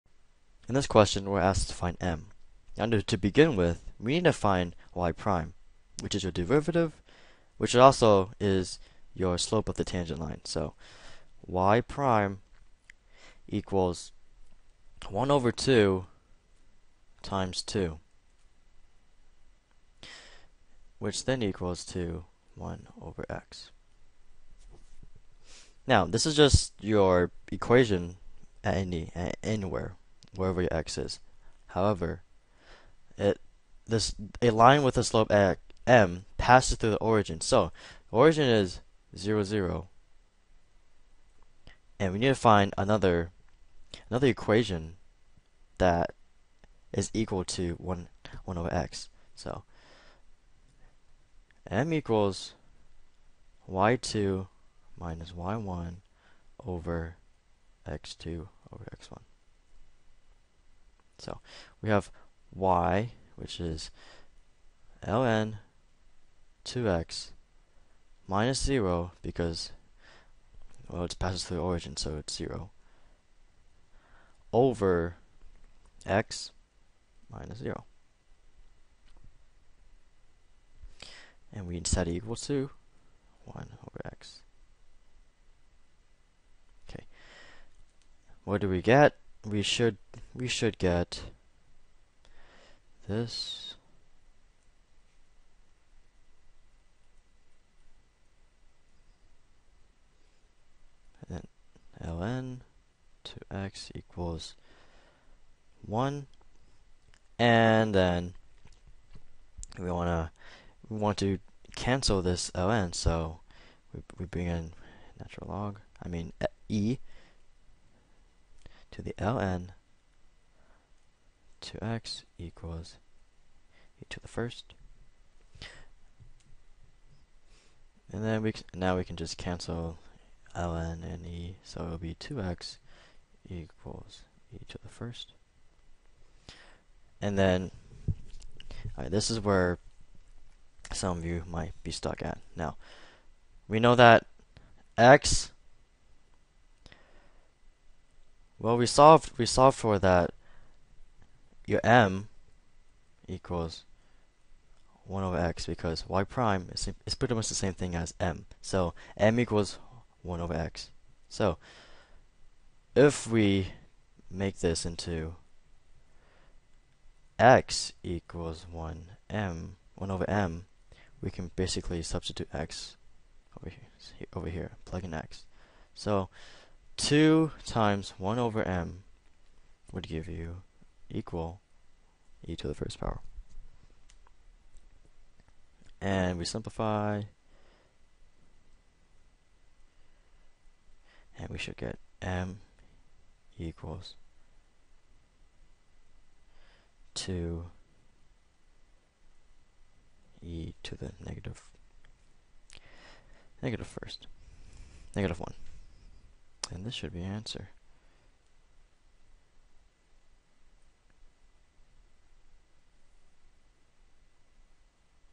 A Tangent through the Origin
A line with slope m passes through the origin and is tangent to y = ln (2x). What is the value of m?